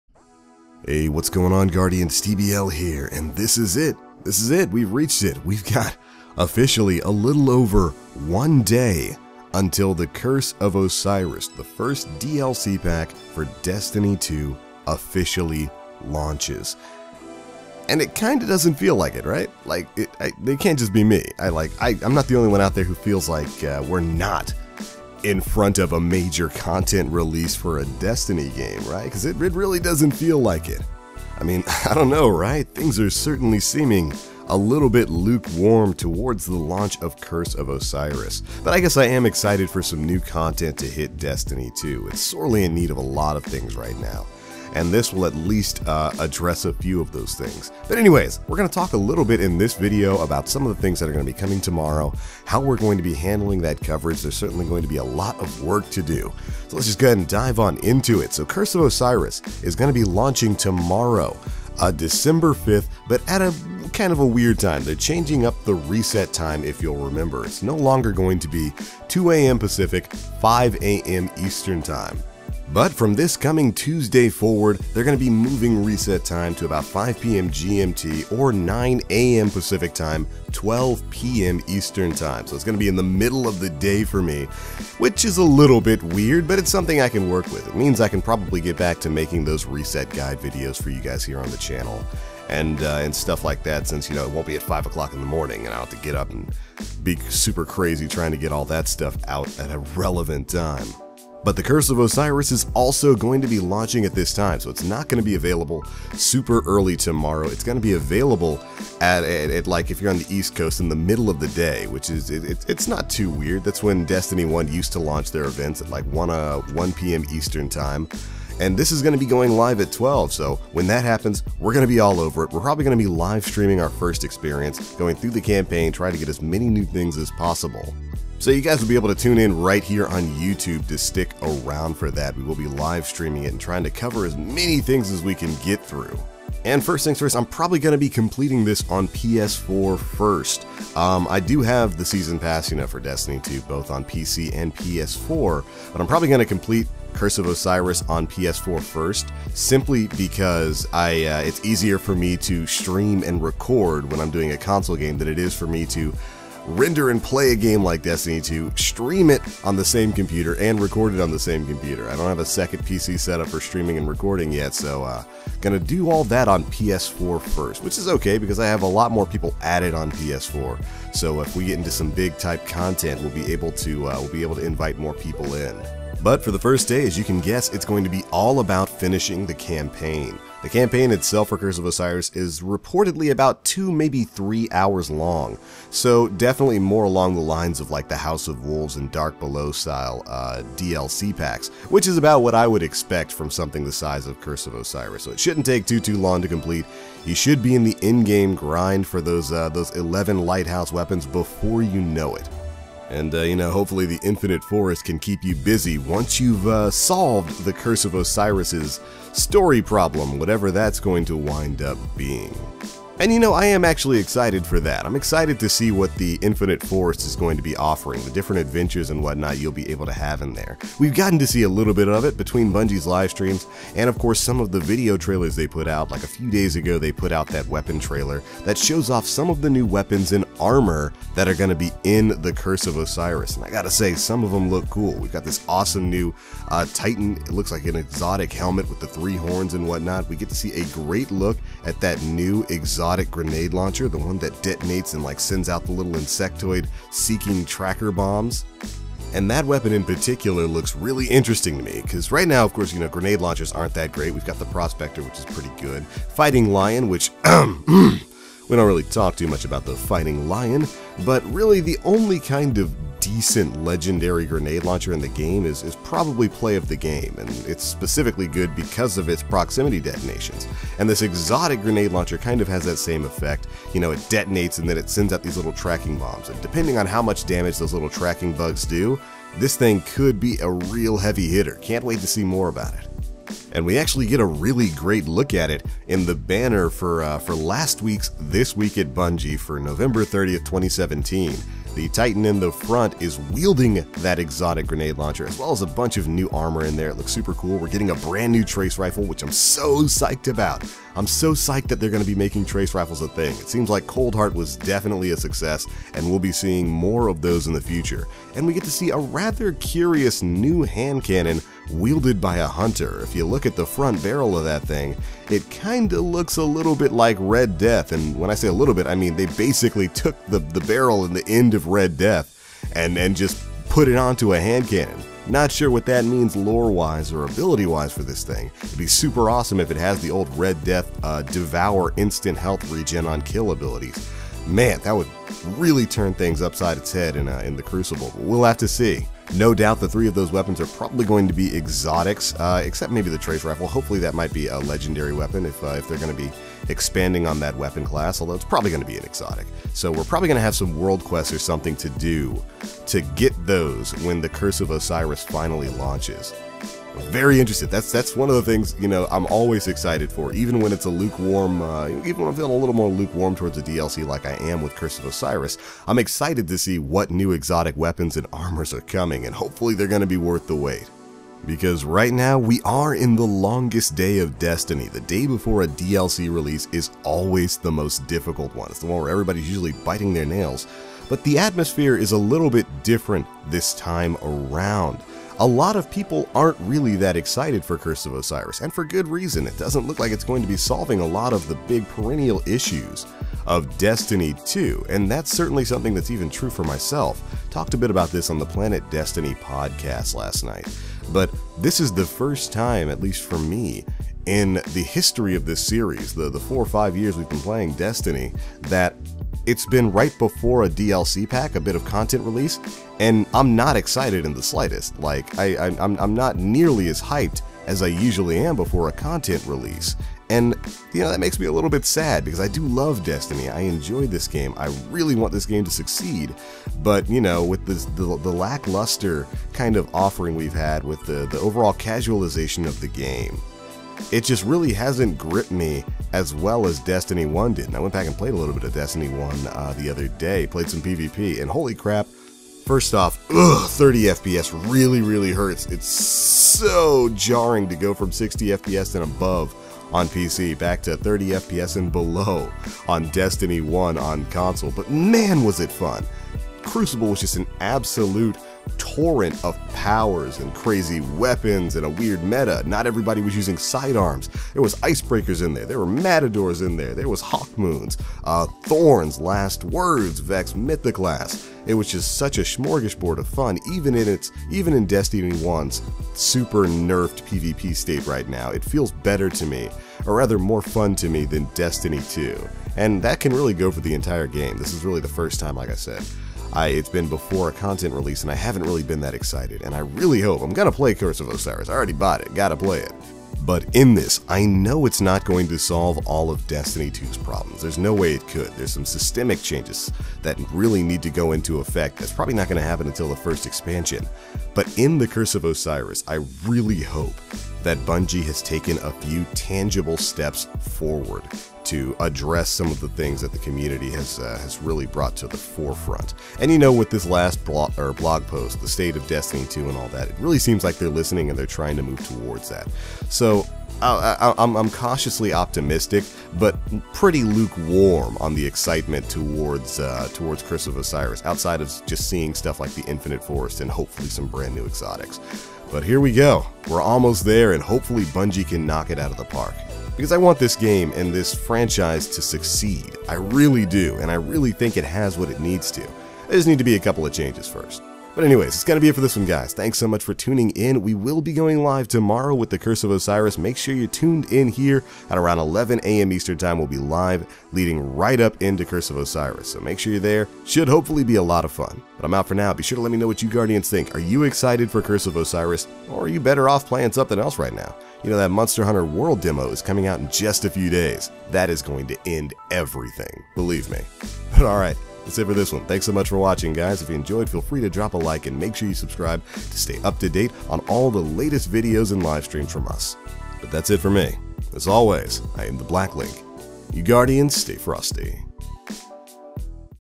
Destiny 2 DLC | New Exotics, Reset Time, & Curse of Osiris Launch Day Plans!
Curse of Osiris drops tomorrow, but are we really all that hyped for it?

Like Bitcoin instead? That's cool:
1Pvb5dGy5vwZpcdiRxcKJtzkRcTfZNED7s

Listen to the EotW Podcast!